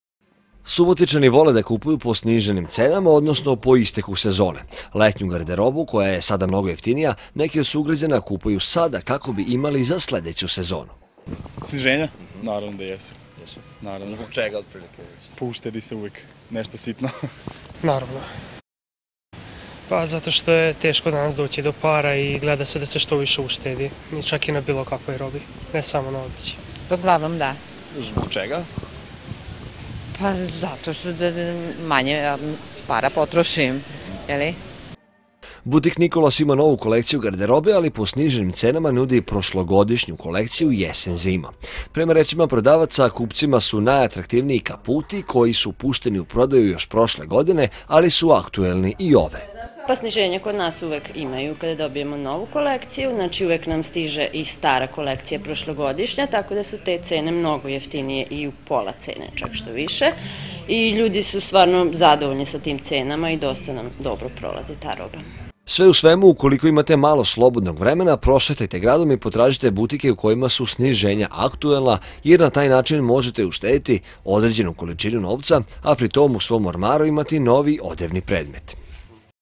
Sniženja u buticima
Sniženja cena garderobe u ovom periodu godine aktuelna su i u većini butika u Subotici. Ova praksa mnogo je rasprostranjenija u zemljama Evropske unije, ali kao i u mnogim stvarima, i u ovim zaostajemo za njima. Kod nas je trenutno jeftinija ili  letnja garderoba, ili modeli zimske garderobe od prošle godine.

Subotičani vole da kupuju po sniženim cenama, odnosno po isteku sezone. Letnju garderobu, koja je sada mnogo jeftinija, neki od sugrađana kupuju sada, kako bi imali za sledeću sezonu.

Butik Nikolas ima novu kolekciju garderobe, ali po sniženim cenama nudi prošlogodišnju kolekciju jesen-zima. Prema rečima prodavaca kupcima su najatraktivniji kaputi, koje su pušteni u prodaju još prošle godine, ali su aktuelni i ove.

Sve u svemu, ukoliko imate malo slobodnog vremena prošetajte gradom i potražite butike u kojima su sniženja aktuelna, jer na taj način možete uštedeti određenu količinu novca, a pri tom, u svom ormaru imati novi odevni predmet.